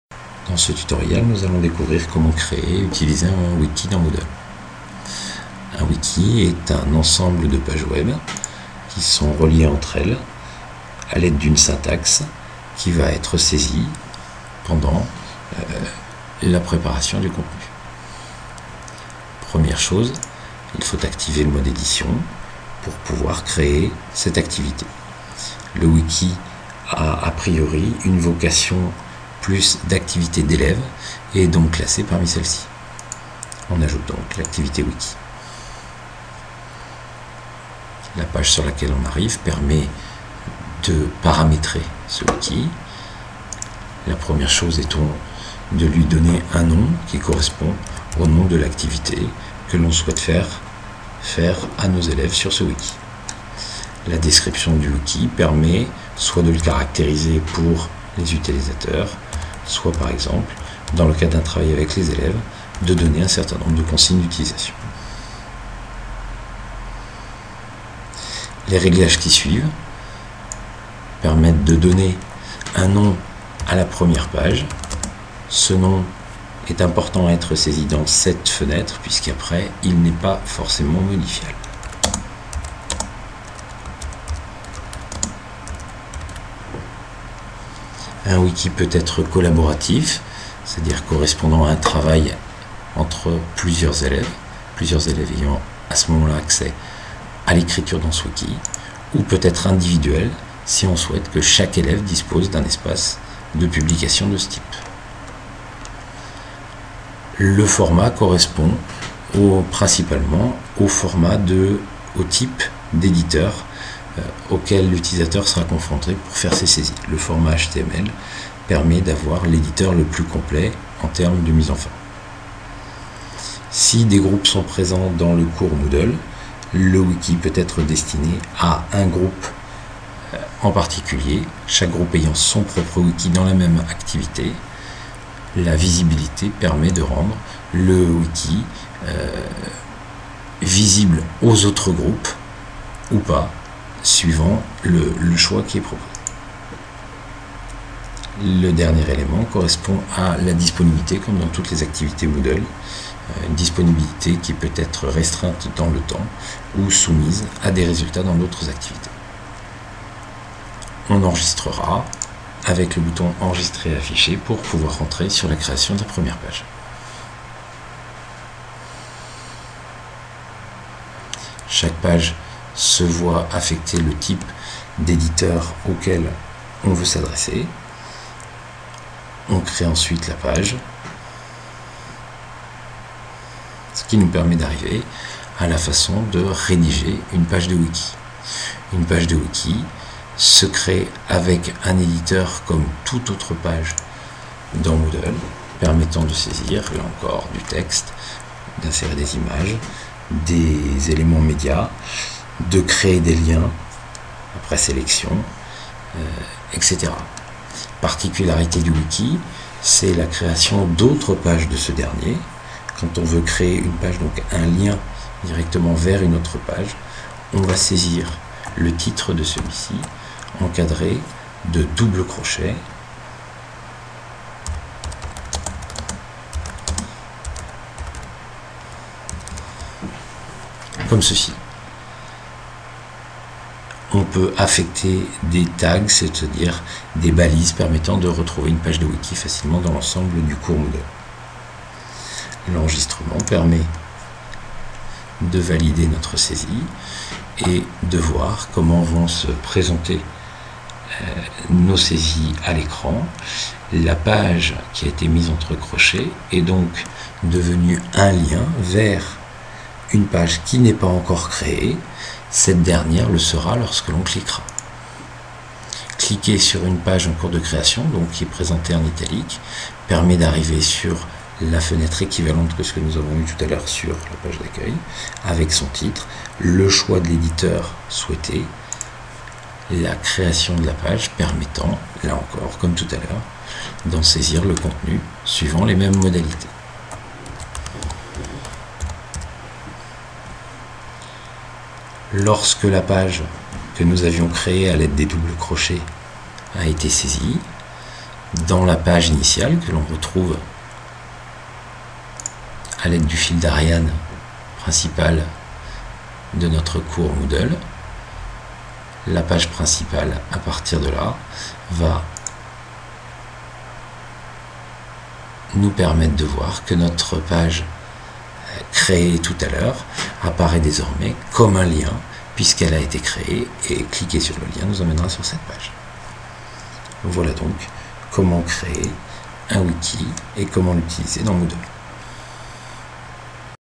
Créer et utiliser un wiki dans moodle 2
Formation à la plateforme Moodle2

Moddle est une plateforme de formation à distance utilisée comme lieu d'activités pédagogiques dans un Espace Numérique de Travail dans le secondaire et le supérieur

Episode : Créér un wiki dans moodle 2